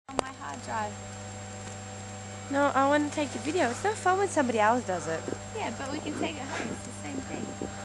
Serre Chevalier nightime dump
Chantelmerle in the Serre Chevalier ski resort in France getting dumped on heavily again, February 2006. Cool effects as the snow hits the light beams.